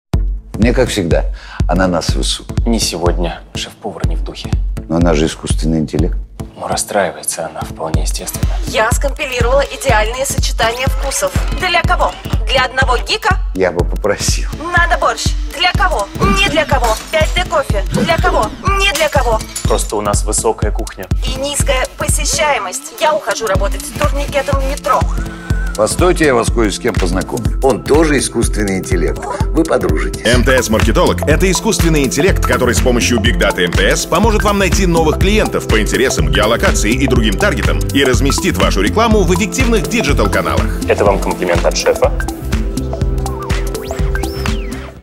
New! MTS / ЛЕОНИД ЯРМОЛЬНИК
Продолжаем публиковать результаты безупречной совместной работы эталонного клиента МТС, агентства @bbdomoscow и ИмперияМузыки artist management (в данном случае) в лице ЛеонидЯрмольник.